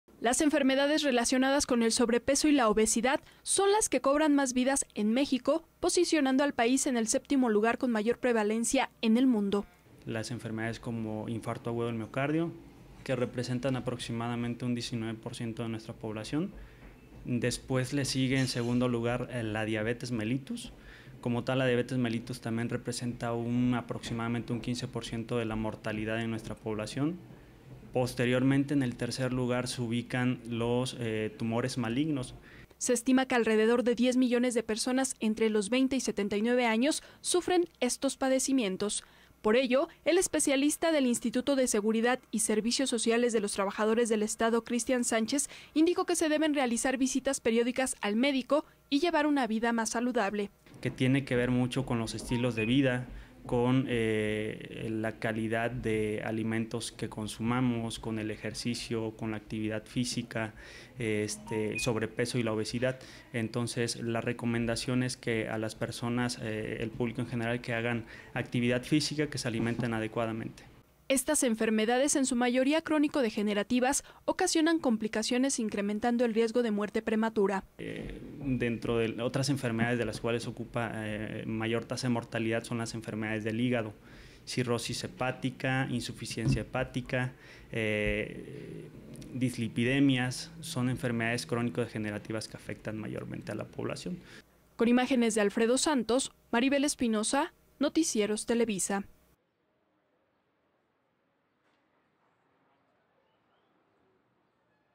Enfermedades con mayor tasa de mortalidad
Las enfermedades del corazón ocupan la primera causa de muerte en el país.